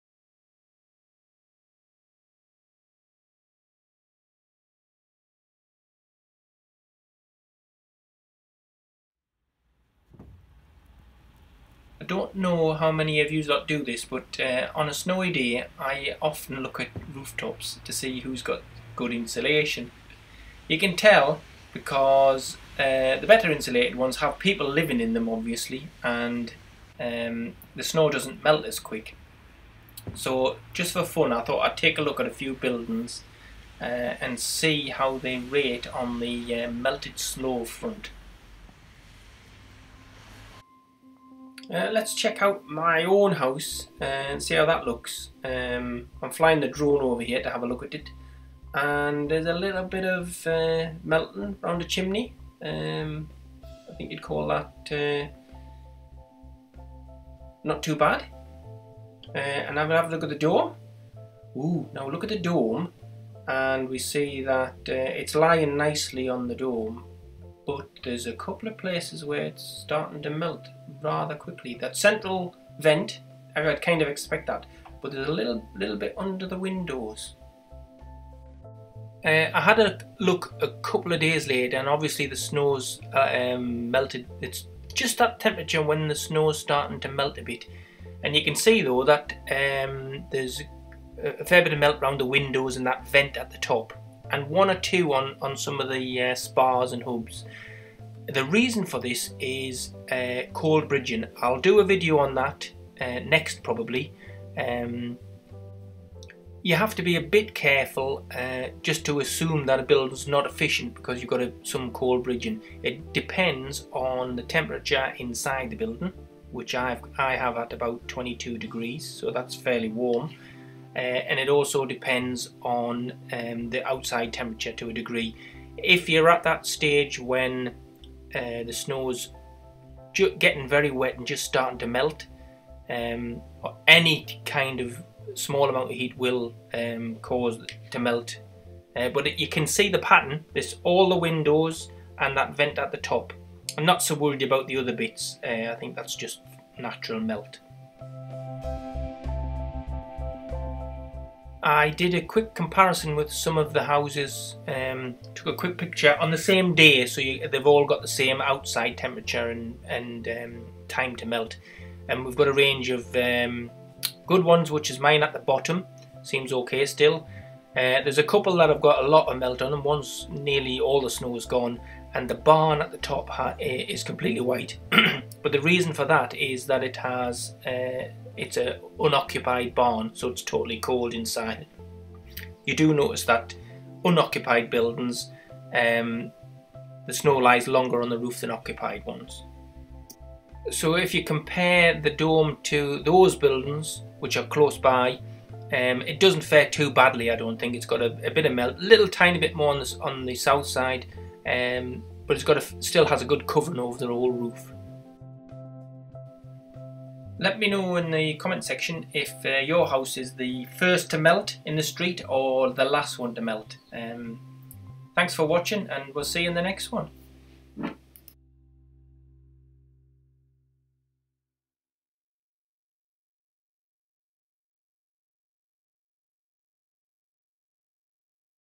Snow melting on your roof = poor insulation
Have you noticed that snow melts quicker on some roofs rather than others, it's a big tell on how good your insulation is.
In todays video we take a quick look at some house roofs and a look at my own dome roof, the dome didn't perform as well as you might think.